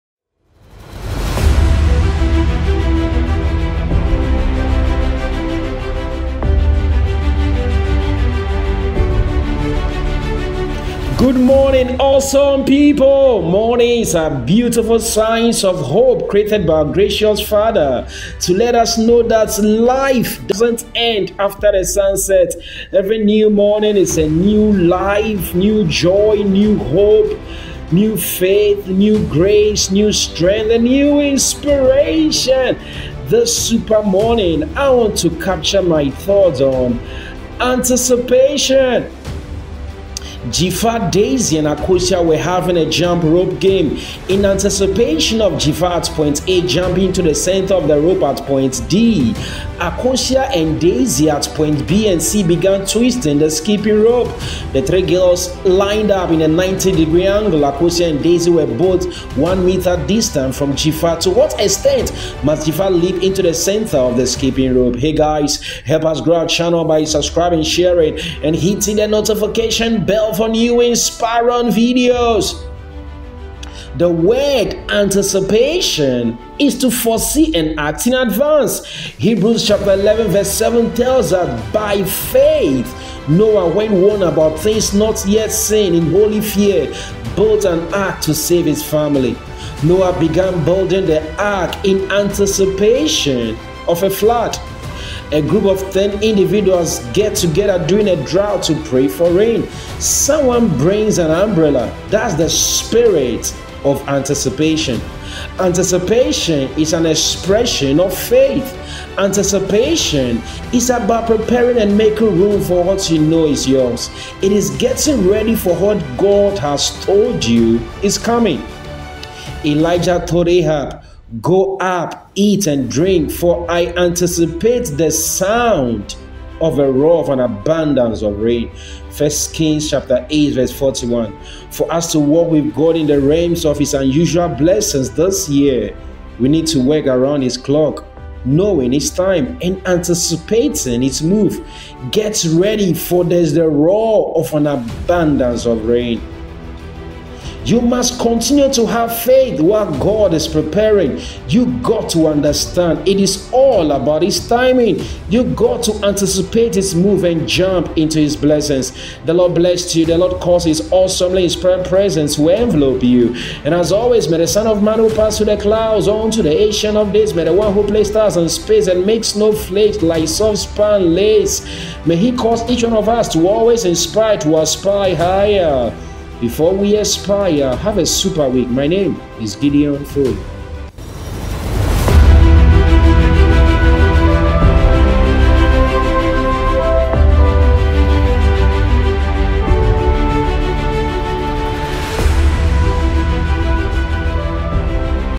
ANTICIPATION
Dzifa, Daisy and Akosua were having a jump rope game. In anticipation of Dzifa at point A jumping into the center of the rope [at point D], Akosua and Daisy at points B and C began twisting the skipping rope. The three girls lined up in a 90-degree angle. Akosua and Daisy were both one meter distant from Dzifa. To what extent must Dzifa leap into the center of the skipping rope?

The word ‘anticipation’ is to foresee and act in advance. Hebrews 11:7 tells us, ‘By faith Noah, when warned about things not yet seen, in holy fear built an ark to save his family.’ Noah began building the ark in anticipation of a flood.

A group of ten individuals get together during a drought to pray for rain. Someone brings an umbrella. That's the spirit of anticipation! Anticipation is an expression of faith. Anticipation is about preparing and making room for what you know is yours. It is getting ready for what God has told you is coming.

Elijah told Ahab, "Go up, eat and drink, for I anticipate the sound of the roar of an abundance of rain." - 1 Kings 18:41. For us to walk with God in the realms of His unusual blessings this year, we need to work around His clock; knowing His time and anticipating His move. Get ready! For there is the roar of an abundance of rain.

You must continue to have faith
While God is preparing
You got to understand
It's all about His timing
You got to anticipate His move
And jump into His blessings

Poet Gideon Foli
GPS: GL-050-5893